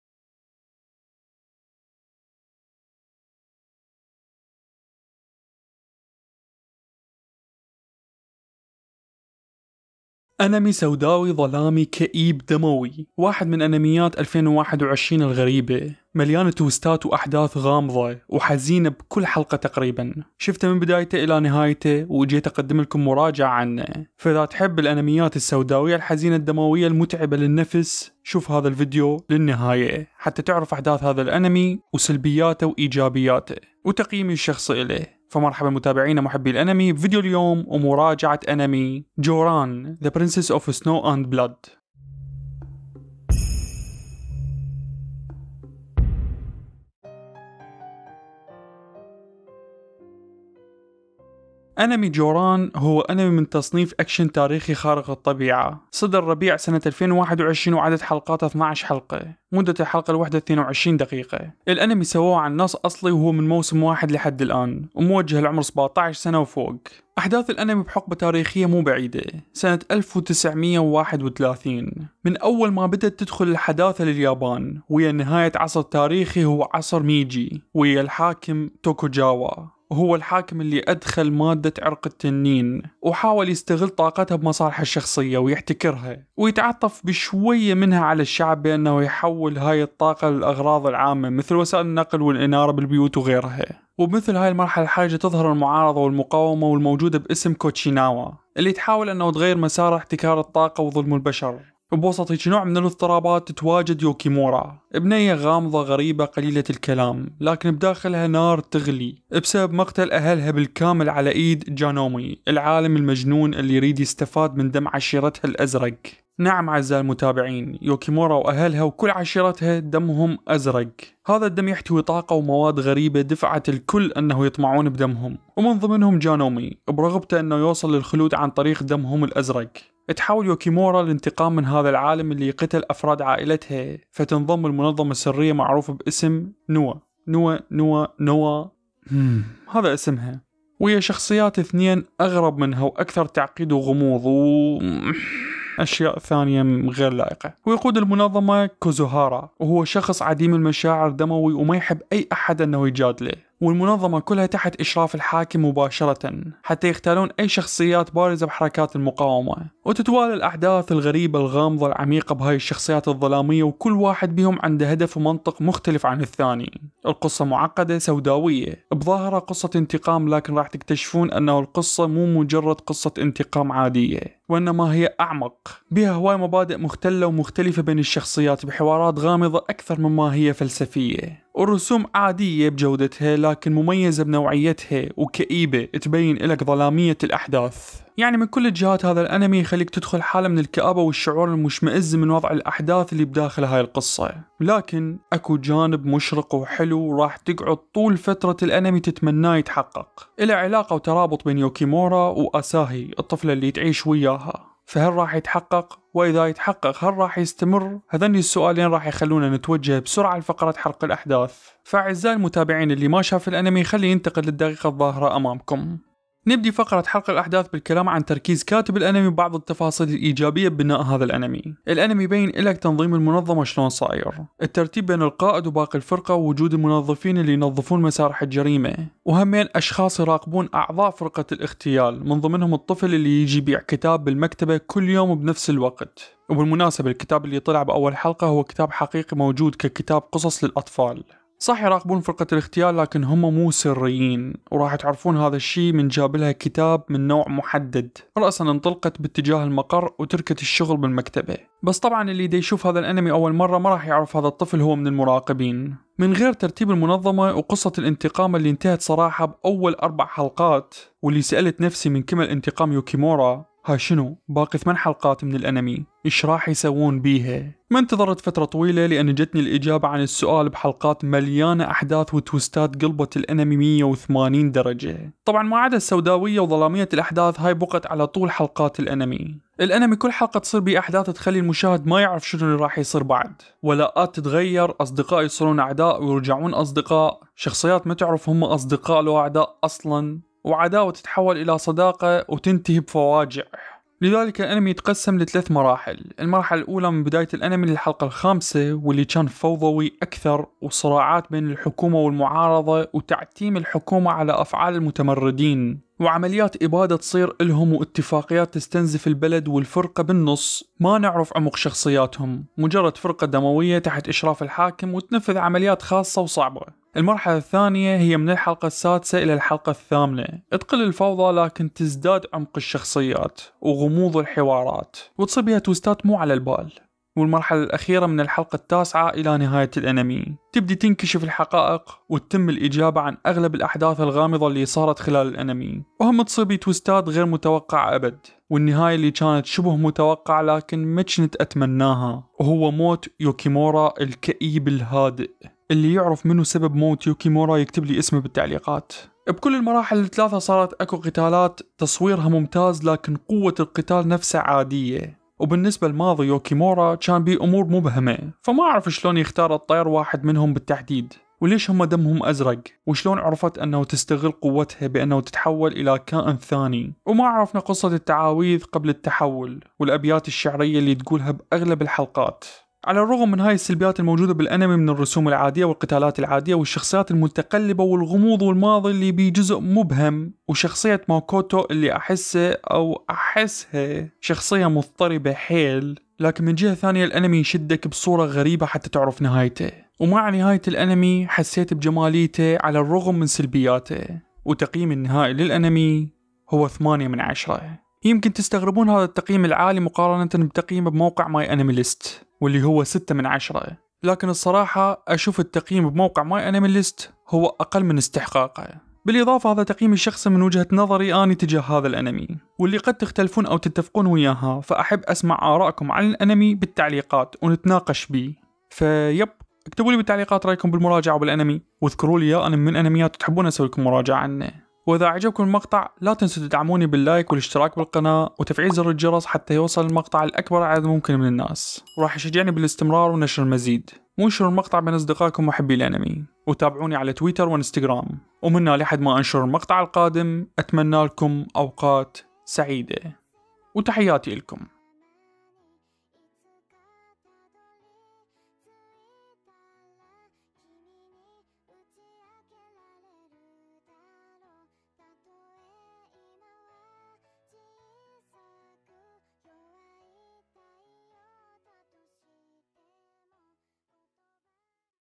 مراجعة | Jouran: The Princess of Snow and Blood
اصدقائي المتابعين اليوم راح اقدم الكم مراجع لانمي سوداوي من انميات 2021
اسم الانمي
Jouran: The Princess of Snow and Blood
المراجعة تحتوي على حرق للاحداث موجود تنبيه عند بداية فقرة الحرق وموجود توقيت نهاية الفقرة داخل المقطع

0:00 انترو
0:46 قصة الانمي
3:35 فقرة حرق الاحداث
6:30 نهاية فقرة الحرق